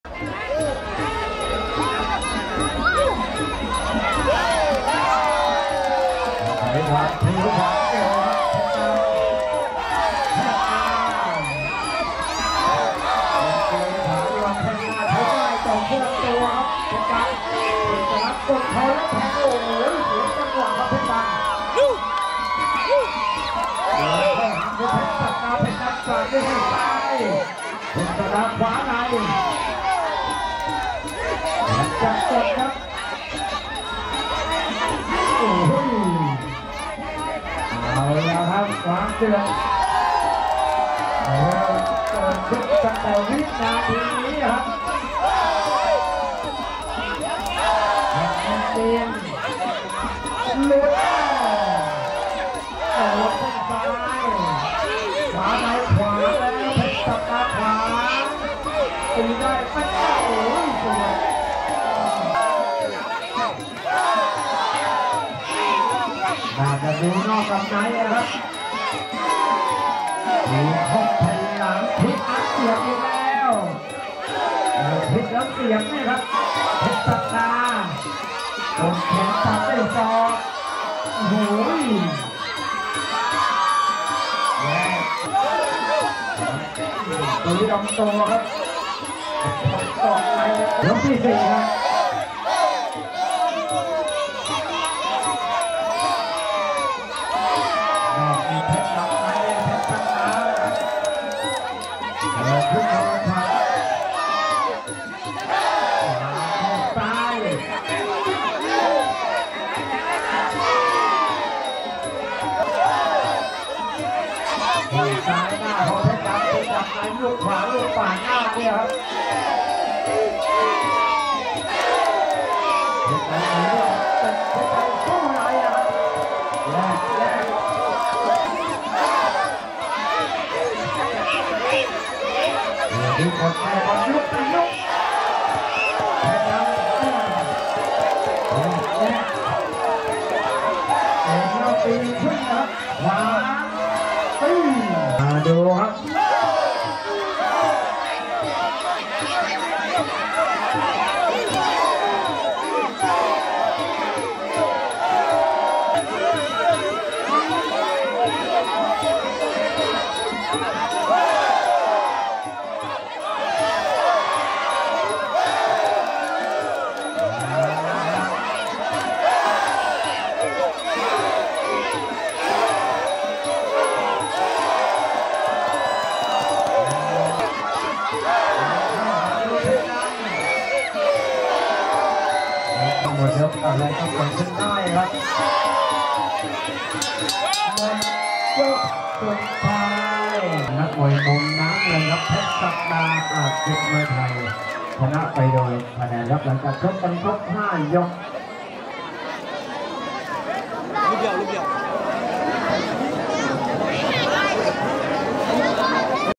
เพชรดำ ศิษย์แสนเชิง vs เพชรศักดา อาจศึกมวยไทย
ไฮไลท์…เพชรดำ ศิษย์แสนเชิง   vs  เพชรศักดา อาจศึกมวยไทย เวทีมวยชั่วคราววัดหนองตาคง ต.หนองบัวศาลา อ.เมือง จะ.นครราชสีมา ( 21 เม.ย.67)